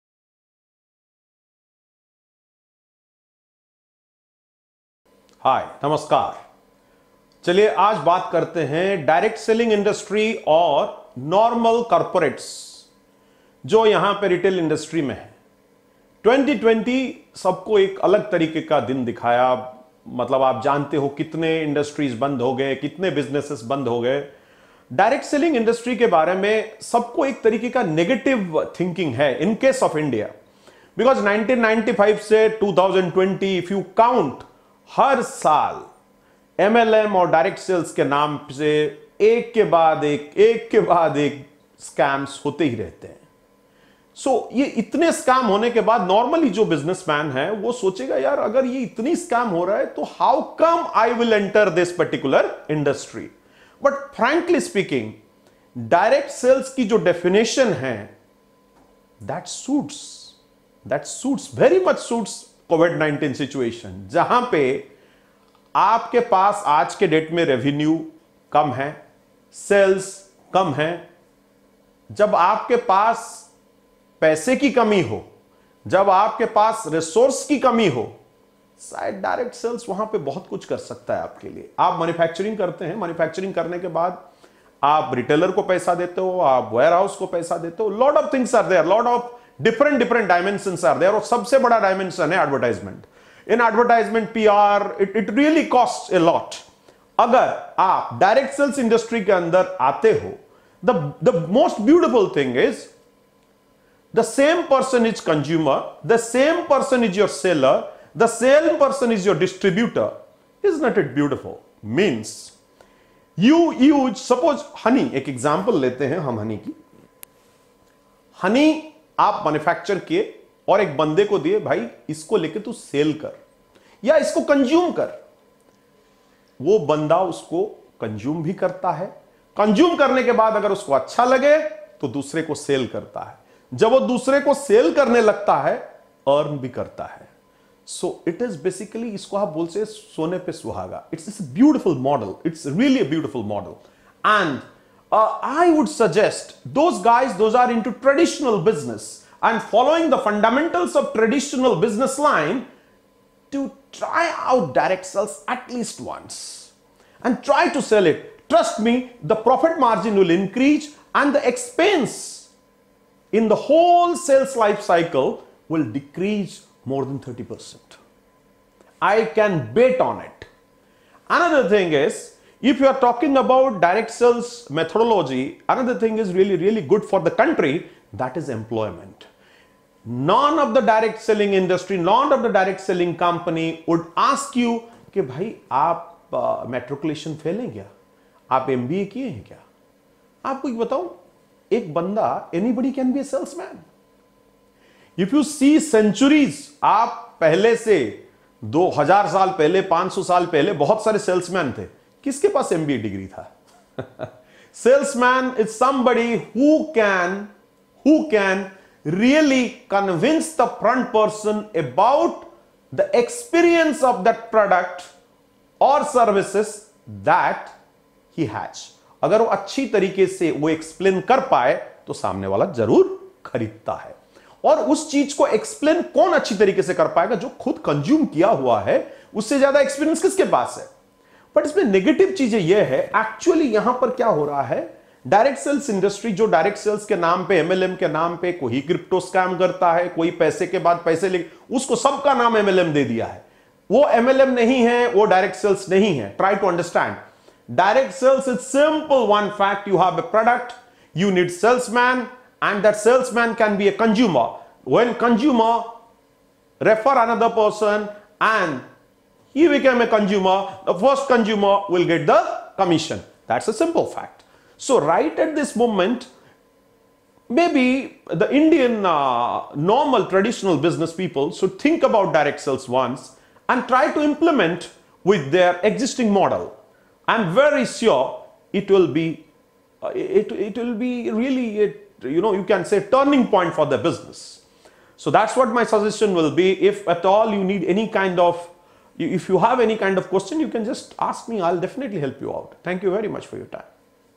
Why Direct Sales Approach is Better than Retail Corporates – Adv. Mohan Mishra | Finlaw Consultancy
Why companies should follow a Direct Sales approach over corporate retail Explained!!

Today, Adv. Mohan Mishra explains why companies should choose the Direct selling approach rather than Corporate strategies, Get all tips and information about Direct selling, MLM Businesses, and other industries in our upcoming videos.

Get all the answers from our video series presented by Finlaw Consultancy Pvt. Ltd.

क्यों कंपनियों को कॉर्पोरेट रिटेल के बारे में प्रत्यक्ष बिक्री के दृष्टिकोण का पालन करना चाहिए !!

आज एड। मोहन मिश्रा बताते हैं कि कंपनियों को कॉरपोरेट रणनीतियों के बजाय डायरेक्ट सेलिंग दृष्टिकोण क्यों चुनना चाहिए, हमारे आने वाले वीडियो में डायरेक्ट सेलिंग, एमएलएम बिजनेस और अन्य उद्योगों के बारे में सभी टिप्स और जानकारी प्राप्त करें।

Finlaw Consultancy Pvt द्वारा प्रस्तुत हमारी वीडियो श्रृंखला के सभी उत्तर प्राप्त करें। लिमिटेड